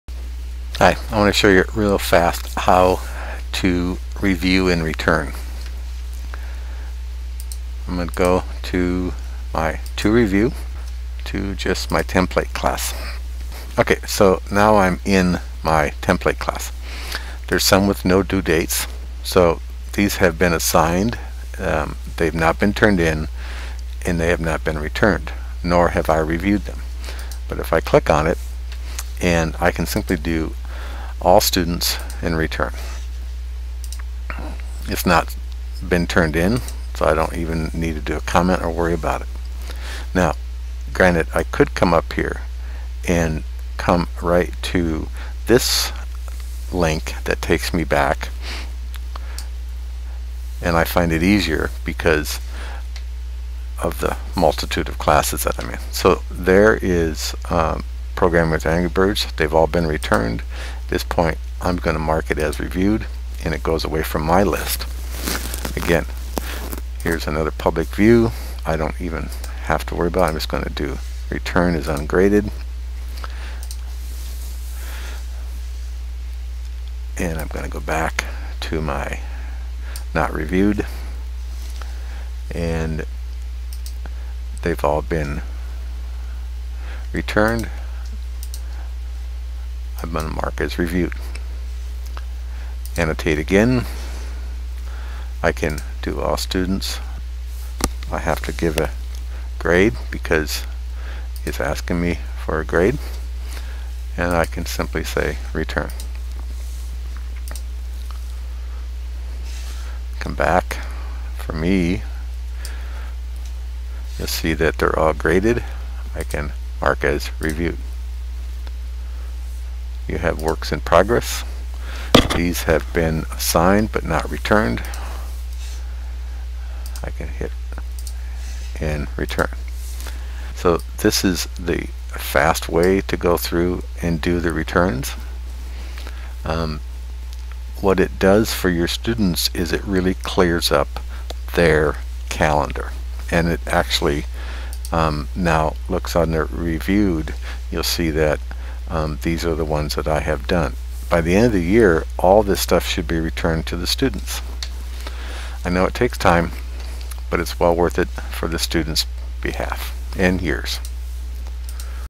Google Classroom Review and Return
Quick video on how to Grade, Return, and Mark as Reviewed in Google Classroom.